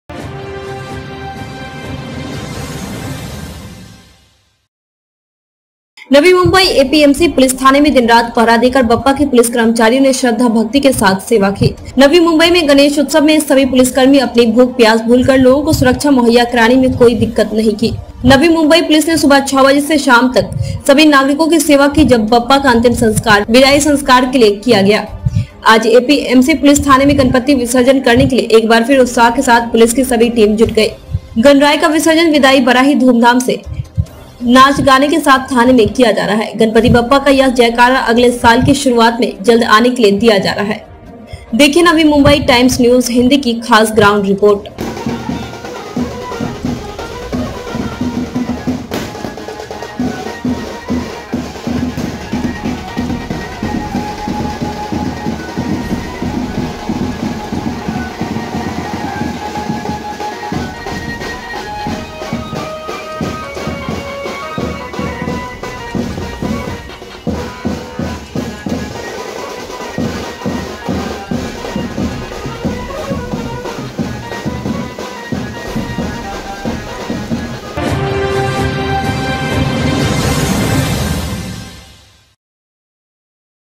Navi Mumbai: एपीएमसी पुलिस थाने में गणपति बप्पा का अंतिम बिदाई धूमधाम से | NMT News की ख़ास रिपोर्ट
परमेश्वर सिंह- एडिटर / नवी मुंबई एपीएमसी पुलिस थाने में दिन-रात पहरा देकर बप्पा की पुलिस कर्मचारियों ने श्रद्धा भक्ति के साथ सेवा की, नवी मुंबई में गणेशोत्सव में सभी पुलिसकर्मी अपनी भूख-प्यास भूलकर लोगों को सुरक्षा मुहैया कराने में कोई दिक्कत नहीं कि। नवी मुंबई पुलिस ने पुलिस ने सुबह छह बजे से शाम तक सभी नागरिकों की सेवा की जब बप्पा का अंतिम बिदाई संस्कार के लिये किया गया। आज एपीएमसी पुलिस थाने में गणपति विसर्जन करने के लिए एक बार फिर उत्साह के साथ पुलिस की सभी टीम जुट गई,  गणराय का विसर्जन विदाई बडा ही धूमधाम नाच गाने के साथ थाने में किया जा रहा है। गणपति बप्पा का यह जयकारा अगले साल की शुरुआत में जल्दी आने के लिए दिया जा रहा है। देखिए नवी मुंबई टाइम्स न्यूज़ हिंदी की खास ग्राउंड रिपोर्ट